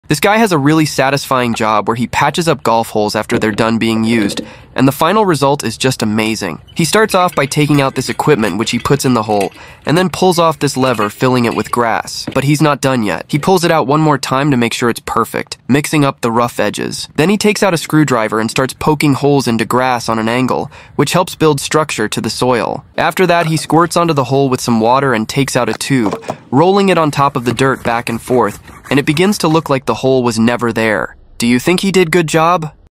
The Most Satisfying Job Ever
This guy fills and replaces golf holes when they are no longer needed

Credits: keenankoss via TT